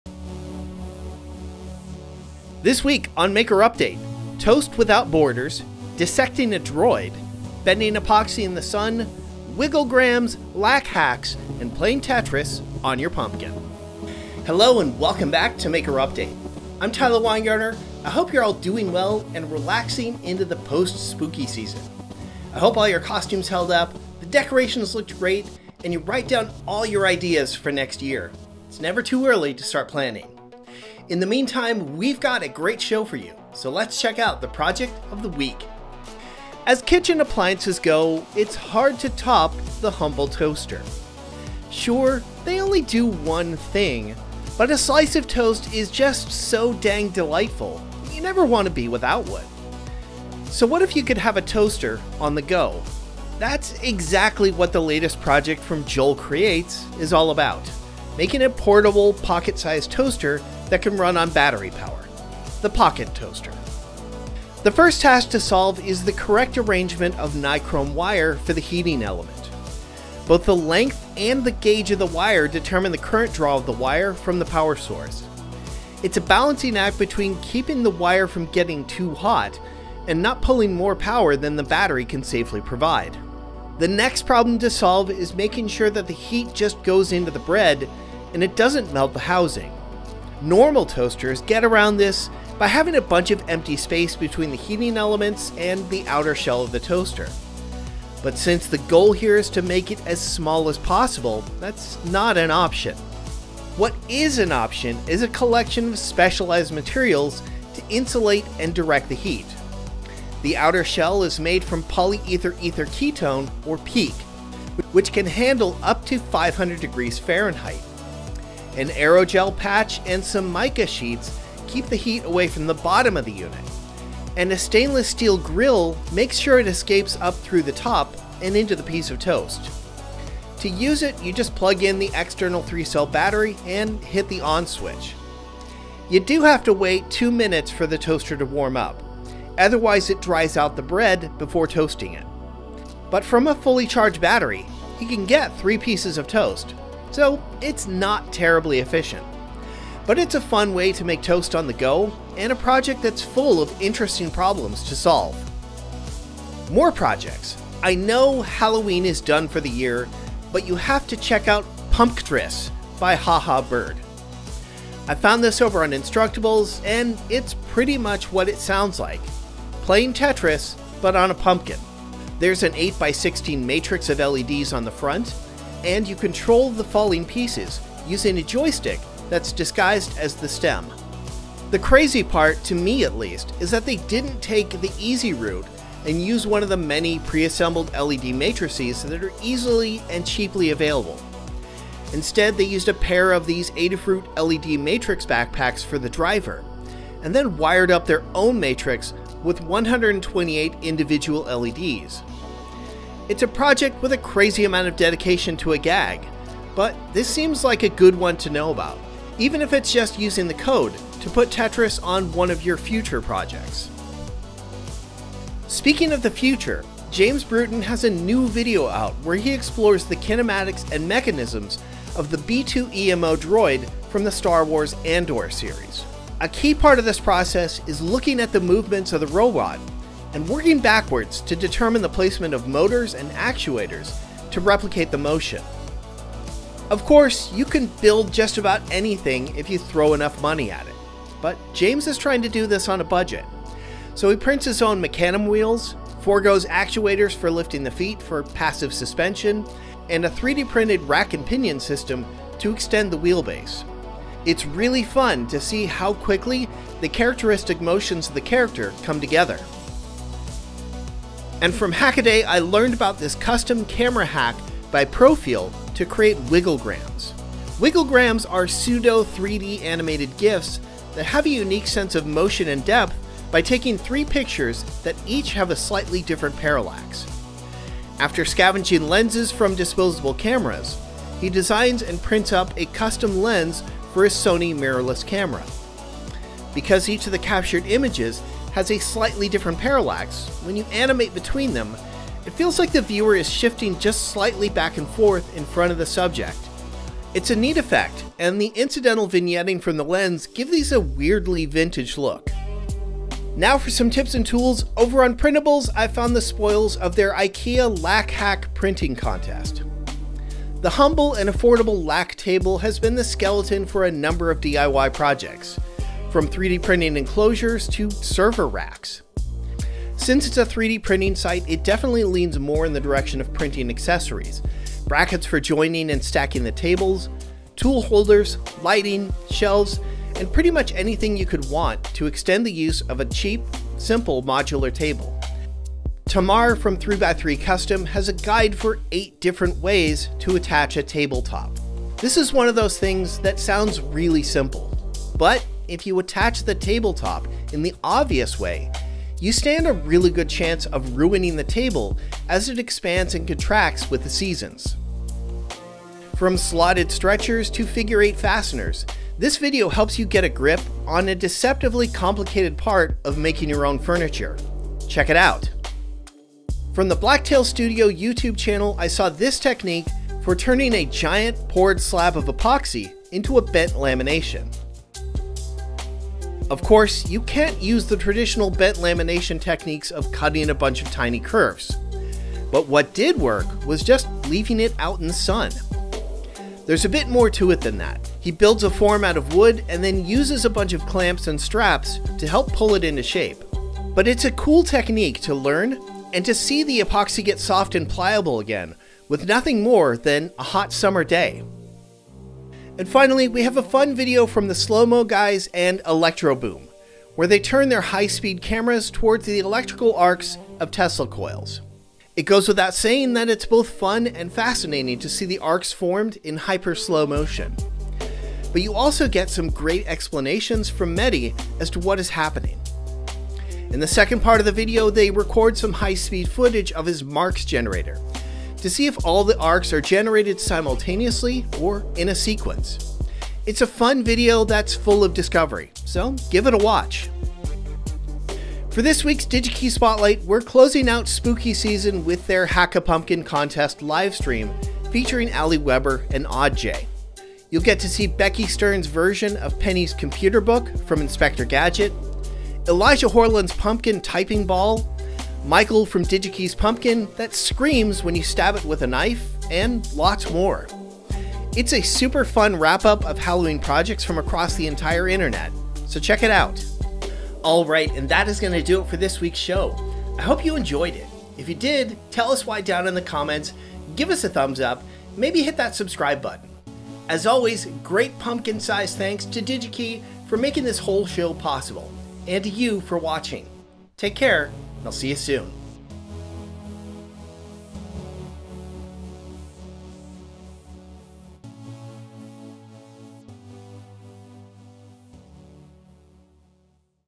Hot Pocket [Maker Update] | Maker.io
This week on Maker Update: toast without borders, dissecting a droid, bending epoxy in the sun, wigglegrams, Lack hacks, and playing Tetris on your pumpkin.

-=More Projects=-

-=Tips & Tools=-

-=Digi-Key Spotlight=-